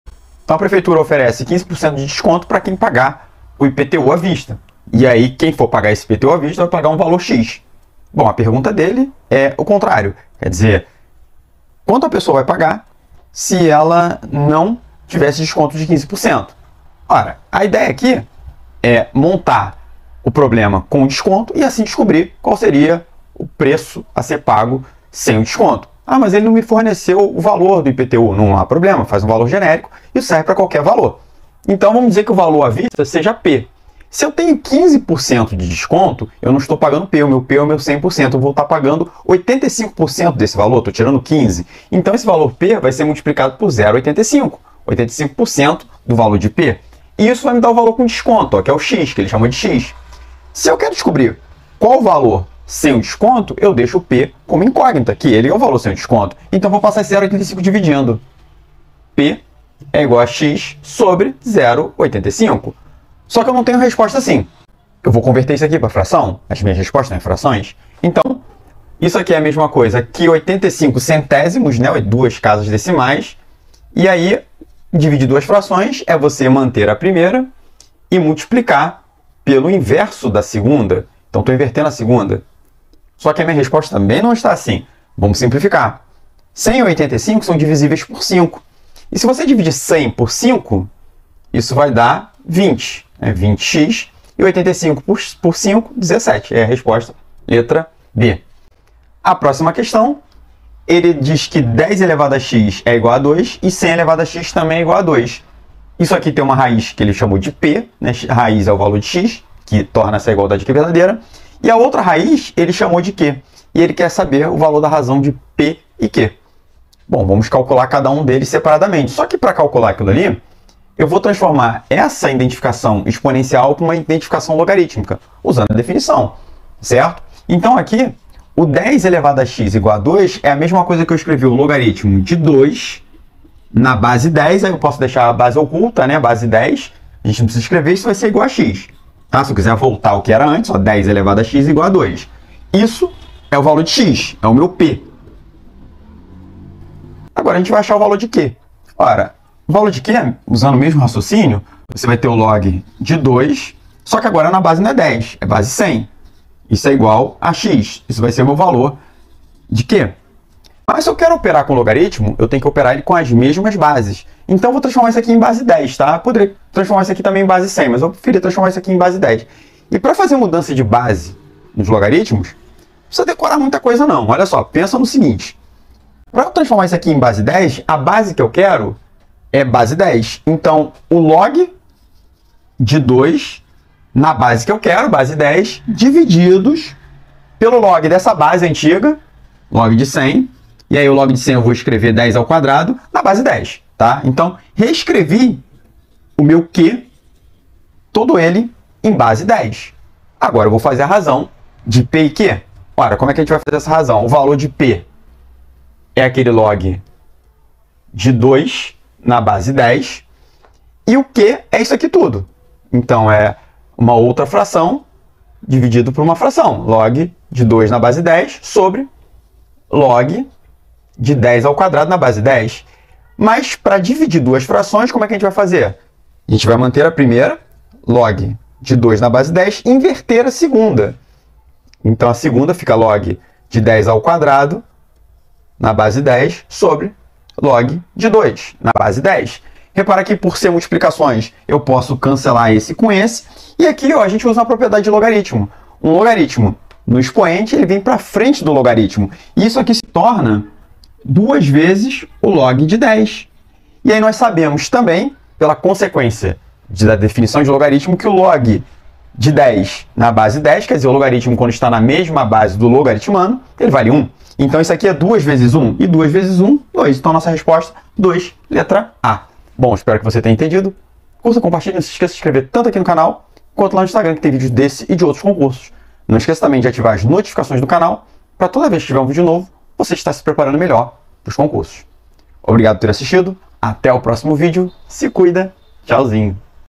Matemática Financeira e Logaritmos
QUESTÃO 17
0:00
Uma prefeitura oferece um desconto de 15% para quem pagar o valor total do IPTU à vista.
O valor total, em reais, sem o desconto do IPTU, é igual a:
(A) 37x/20
(B) 20x/17
(D) 3x/2

QUESTÃO 18
1:38
As equações exponenciais  10ˣ = 2 e 100ˣ = 2 possuem raízes respectivamente iguais a p e q.
O valor de p/q é igual a:
(A) 2
(B) 1
(D) 1/10